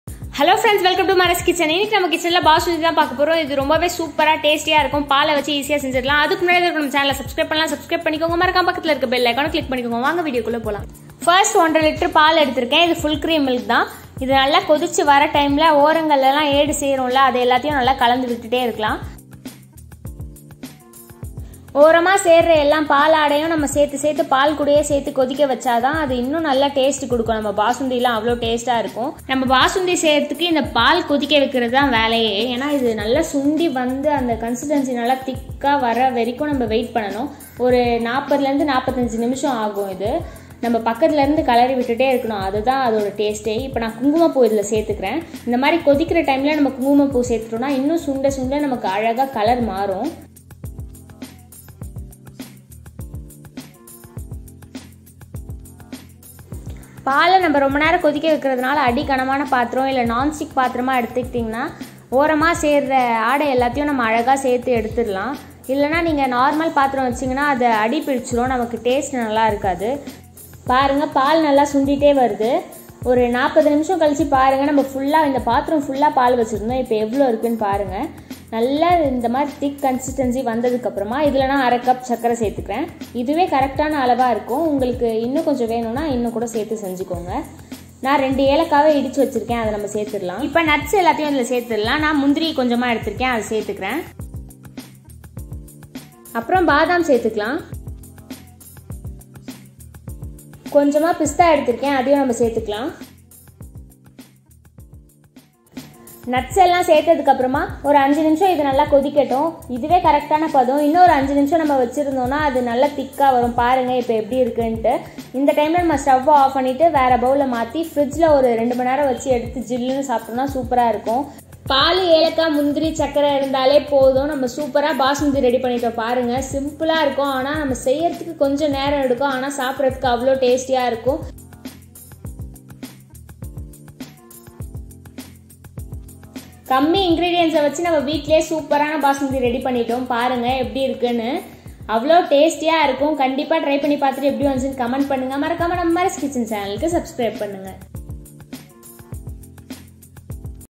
Basundi Recipe in Tamil/Milk Recipe/Sweet Recipe
In this video how to make Basundi try this recipe and please comment your feedback about the recipe

Milk
Sugar
Cardamom
Dry Fruits